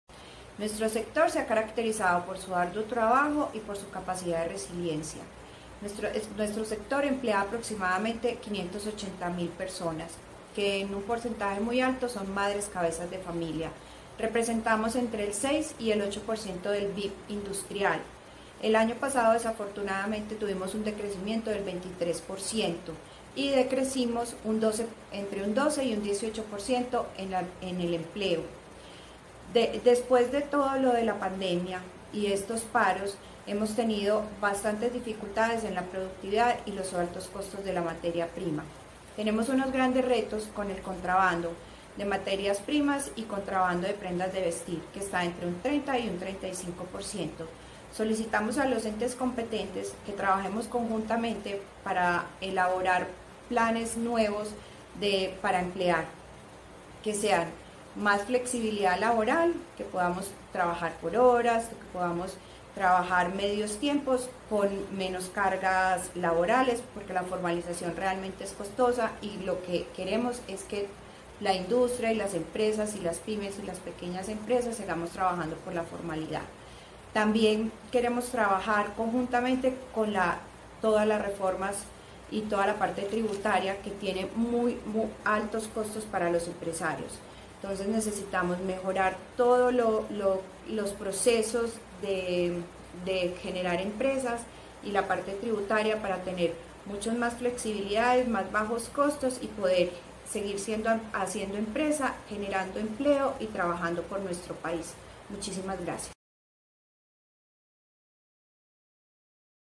Preocupación en el sector Textil por inconvenientes en la producción y altos costos en materia prima
Lina María Vélez Cardona, Presidente sector de Fabricantes de Fenalco Antioquia